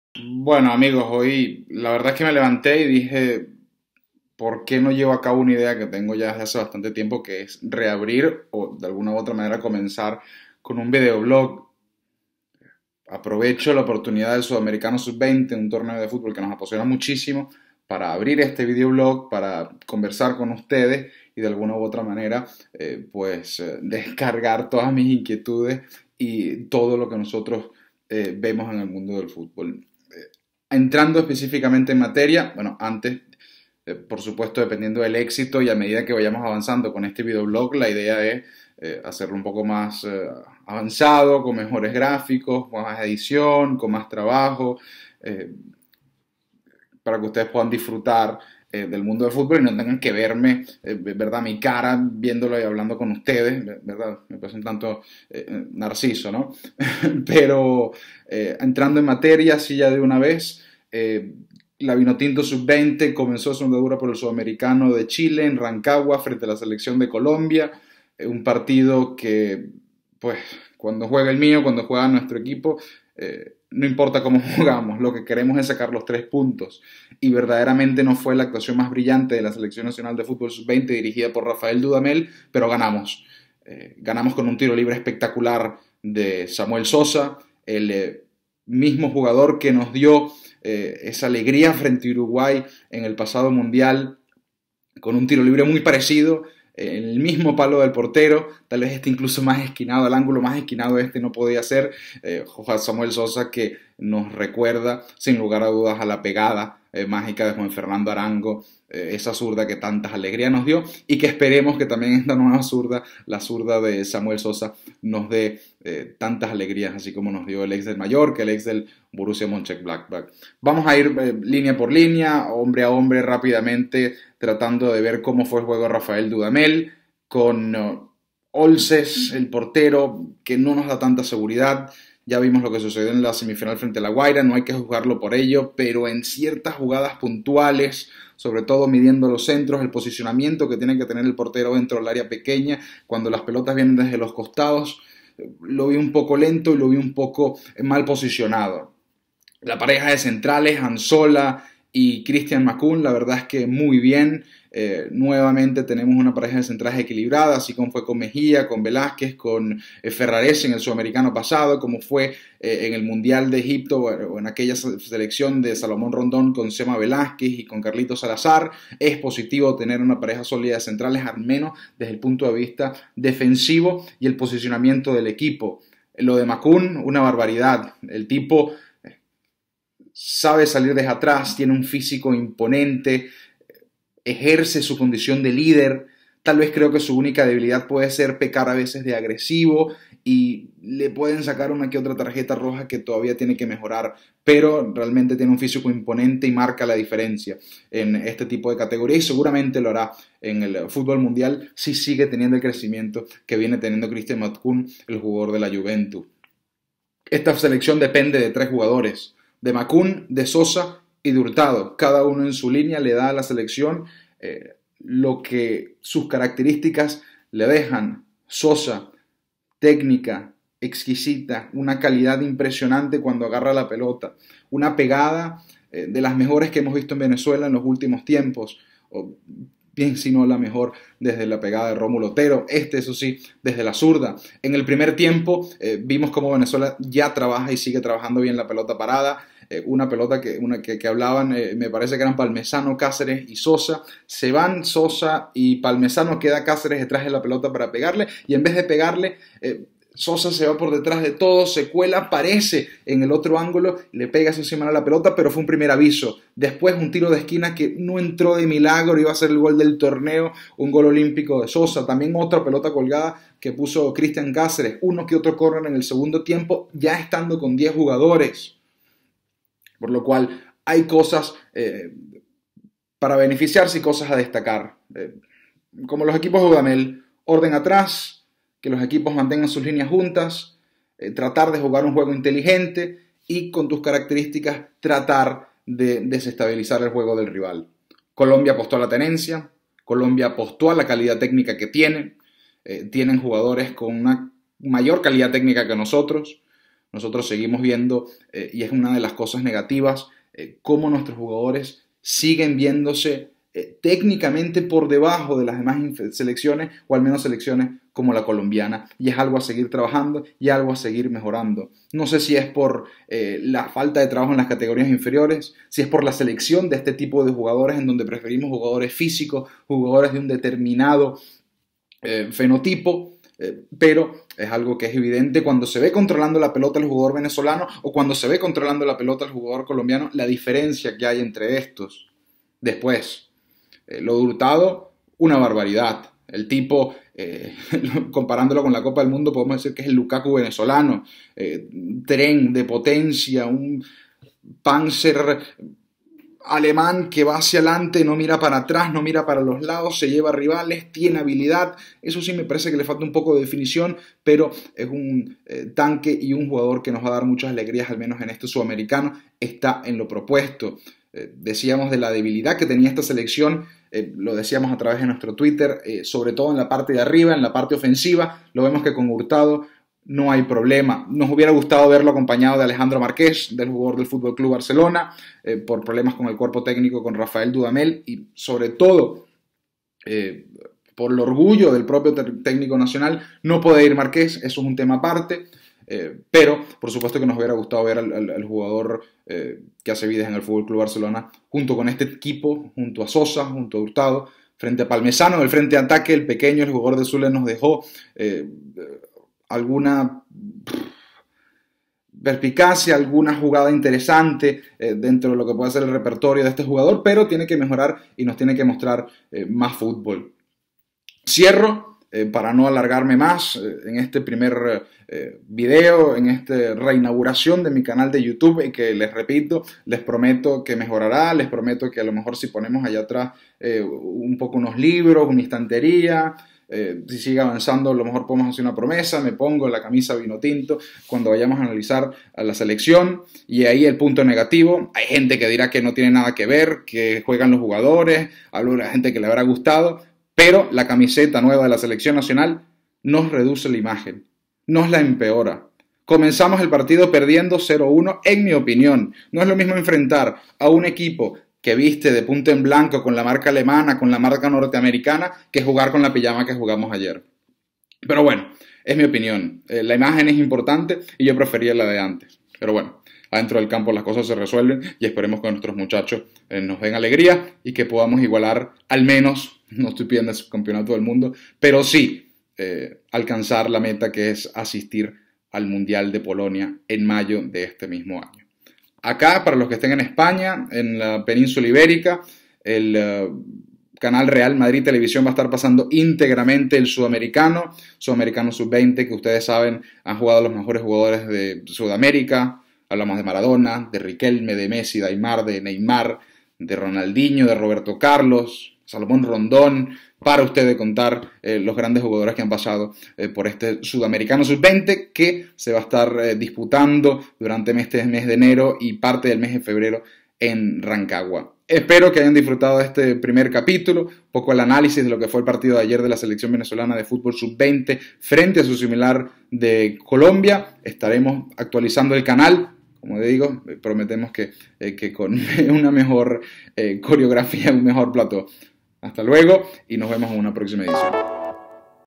Venezuela vs Colombia | Sudamericano Sub 20 2019
He aquí el relanzamiento de mi canal de Youtube.

En esta primera entrega analizaremos el partido Venezuela vs Colombia correspondiente al Sudamericano Sub20 2019.

¡Comenta abajo!

Entrevista zona mixta con Rafael Dudamel Euskadi vs Venezuela